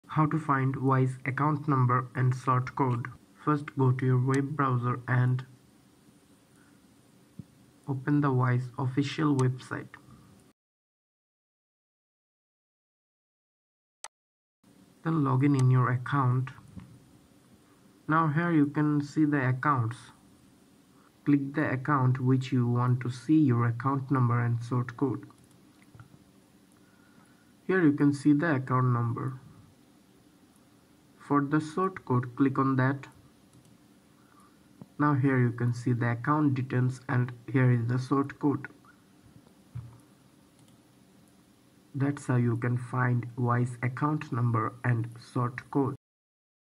How to Find WISE account number and sort code l Double Z
Unlock the simplicity of managing your international finances with our comprehensive guide on "How to Find WISE Account Number and Sort Code," tailored for users in the USA, UK, Canada, and Europe. Whether you're navigating global transactions or simply managing your Wise (formerly TransferWise) account, this tutorial provides step-by-step instructions on locating both your account number and sort code. Learn essential tips for seamless money transfers and stay in control of your financial interactions worldwide. Watch now to empower yourself with the knowledge to effortlessly find your Wise account details, optimizing your international banking experience. Elevate your financial journey with this user-friendly guide.